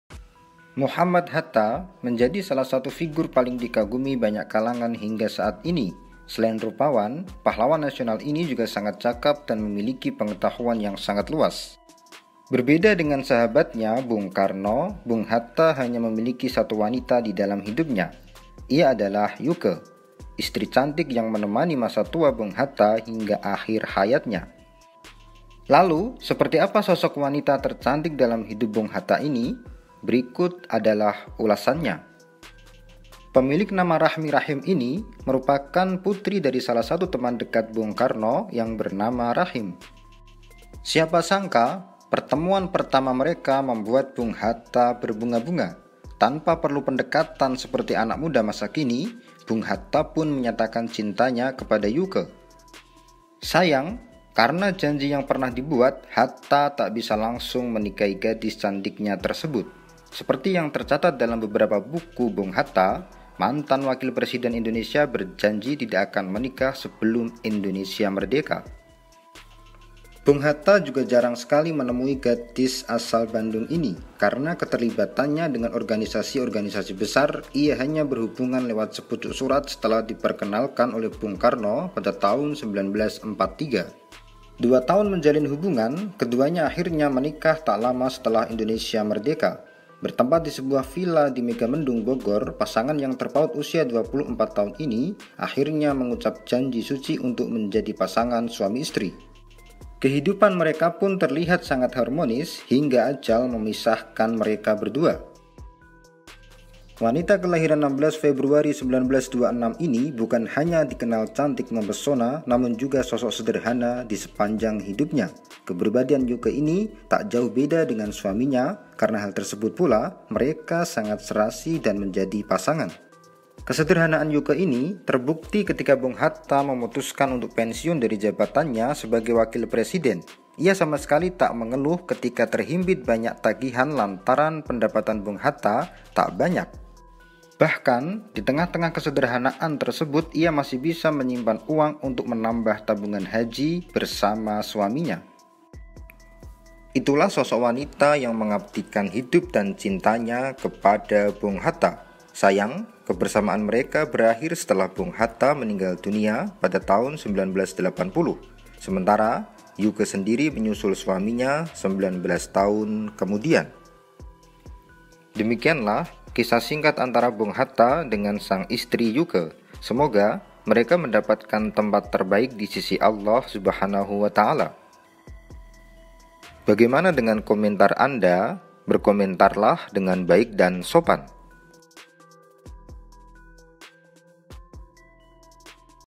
Istri Bung Hatta, Wanita Cantik & Sederhana Dibalik Suksenya Bung Hatta
Muhammad Hatta menjadi salah satu figur paling dikagum-kagumkan banyak kalangan hingga saat ini. Selain rupawan, pahlawan nasional ini juga sangat cakap dan memiliki pengetahuan yang sangat luas. Berbeda dengan sahabatnya bung karno , Bung Hatta hanya memiliki satu wanita di dalam hidupnya. Ialah Yuke, istri cantik yang menemani masa tua Bung Hatta hingga akhir hayatnya. Lalu seperti apa sosok wanita tercantik dalam hidup Bung Hatta ini? Berikut ulasan lengkapnya!
Pemilik nama Rachmi Rahim ini merupakan putri dari salah satu teman dekat Bung Karno bernama Rahim. Siapa sangka, pertemuan pertama membuat Bung Hatta berbunga-bunga. Tanpa perlu pendekatan seperti anak muda masa kini, Bung Hatta pun menyatakan cintanya kepada Yuke. Sayang, karena janji yang pernah dibuat, Hatta tak bisa langsung menikahi gadis cantiknya tersebut. Seperti yang tercatat dalam beberapa buku Bung Hatta, mantan wakil presiden Indonesia berjanji tidak akan menikah sebelum Indonesia Merdeka
, Bung Hatta juga jarang sekali menemui gadis asal Bandung ini. Karena keterlibatannya dengan organisasi-organisasi besar Ia hanya berhubungan lewat sepucuk surat setelah diperkenalkan oleh Bung Karno pada tahun 1943. Pun kisah asmara mereka hanya terhubung lewat surat, siapa sangka banyak orang yang terkagum-kagum dengan kesetiaan mereka.
Dua tahun menjalin hubungan, keduanya akhirnya menikah tak lama setelah Indonesia merdeka. Bertempat di sebuah vila di Megamendung, Bogor, pasangan yang terpaut usia 24 tahun ini akhirnya mengucap janji suci untuk menjadi pasangan suami istri. Kehidupan mereka pun terlihat sangat harmonis hingga ajal memisahkan mereka berdua.
Wanita kelahiran 16 Februari 1926 ini bukan hanya dikenal cantik memesona, namun juga sosok sederhana di sepanjang hidupnya. Kepribadian Yuke ini pun tak jauh beda dengan suaminya. Karena hal tersebut pula, mereka sangat serasi dan menjadi pasangan.
Kesederhanaan Yuke ini terbukti ketika Bunga Hatta memutuskan untuk pensiun dari jabatannya sebagai wakil presiden. Ia sama sekali tak mengeluh ketika terhimpit banyak tagihan lantaran pendapatan Bung Hatta tak banyak. Bahkan di tengah-tengah kesederhanaan tersebut ia masih bisa menyimpan uang untuk menambah tabungan naik haji bersama suaminya.
selain itu yuke juga setia menemani suaminya ketika bung hatta ditangkap dan dibuang ke bangka Bukan hanya ditahan, ia juga menyaksikan suaminya disiksa oleh penjajah. Tak hanya itu, Yuke juga ikut merasakan pahitnya kehidupan Bung Hatta ketika ditangkap hingga dibuang ke Bangka.

Di sepanjang kehidupannya, Yuke malah banyak mendapatkan cobaan dari mulai hidup sederhana dengan bayaran Rp 3000 per bulan hingga menjadi tahanan bersama sang suaminya.
selain itu yuke juga setia menemani suaminya ketika bung hatta ditangkap dan dibuang ke bangka .Ya, Yuke pernah menjadi salah satu tahanan tentara Belanda pada akhir tahun 1945. Bukan hanya ditahan, ia juga menyaksikan suaminya disiksa oleh penjajah. Tak hanya itu, Yuke juga ikut merasakan pahitnya kehidupan Bung Hatta ketika ditangkap hingga dibuang ke Bangka.
Itulah sosok wanita yang mengabadikan hidup dan cintanya kepada Bung Hatta. Sayang, kebersamaan mereka berakhir setelah Bung Hatta meninggal dunia pada tahun 1980. Sementara Yuke sendiri menyusul suaminya 19 tahun kemudian.